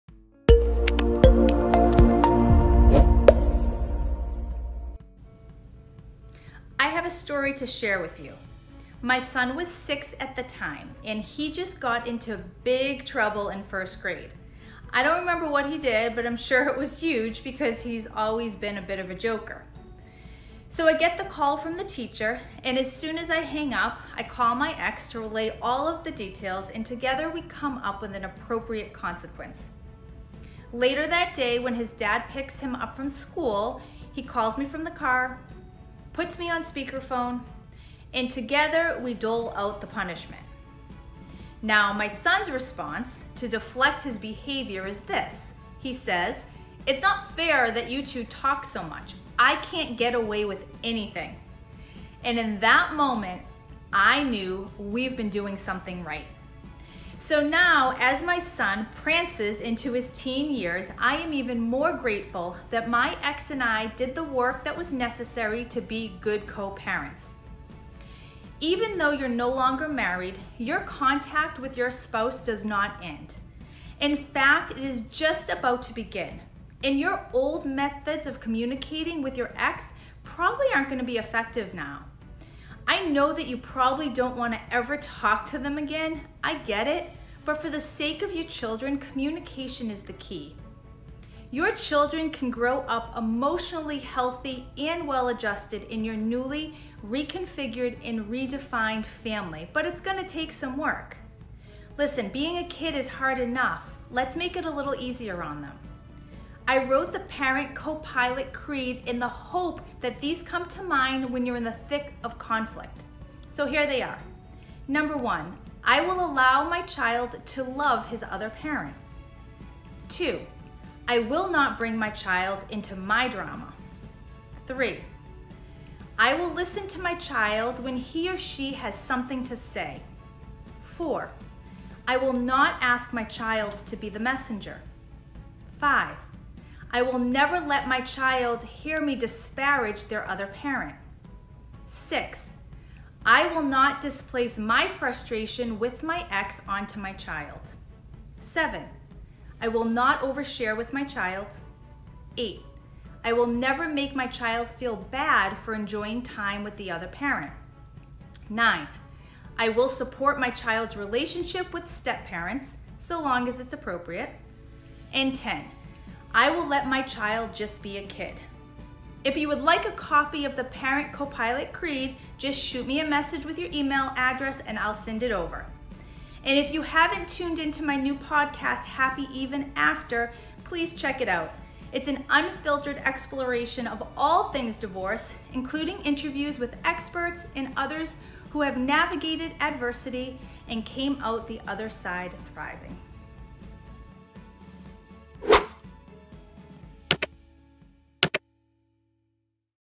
The Parent Co-Pilot Creed | Co-parenting | Renee Bauer
The Parent Co-Pilot Creed: In this video know how to deal with co-parenting with Renee Bauer.
Even though you're no longer married, your contact with your spouse doesn't end. In fact, communication is key if you have kids! They deserve parents who are willing to communicate and keep their best interests at heart.

I wrote the Parent Co-Pilot Creed to help parents cope with changes while also maintaining a healthy balance for your kids. These rules to live by have helped me and several of my clients, as we navigate a co-parenting plan! I hope they help you, as well.

Enrolling NOW for the D-Course!